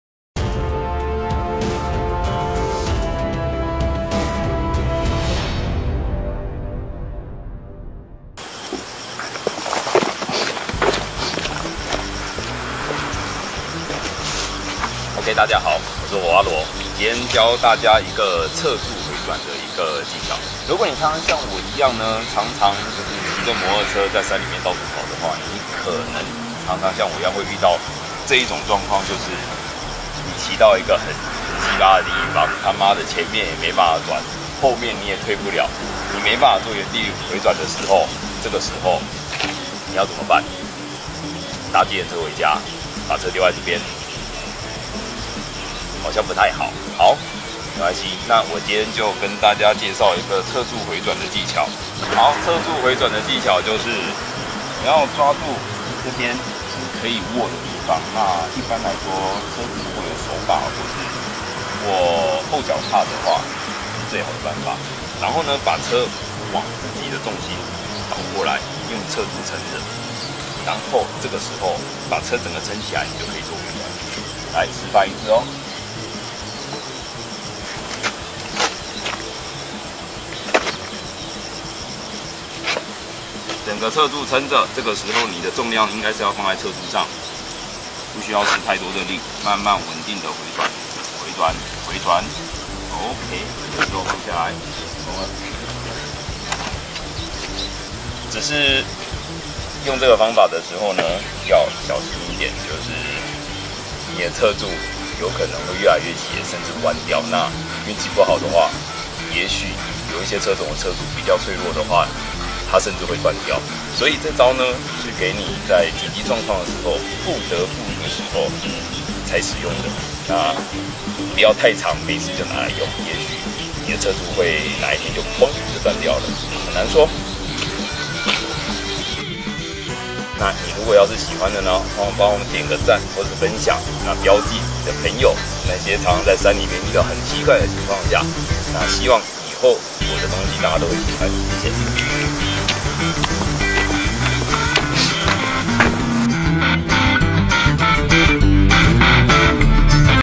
騎到狹路卡住怎麼辦？老司機教你用側柱原地迴轉技巧！『火花羅的騎車玩樂#18』
歡迎大專院校公司行號邀約道安講座
贊助我們一起為交通努力：

Podcast

火花羅的podcast『公路漂流』開張啦！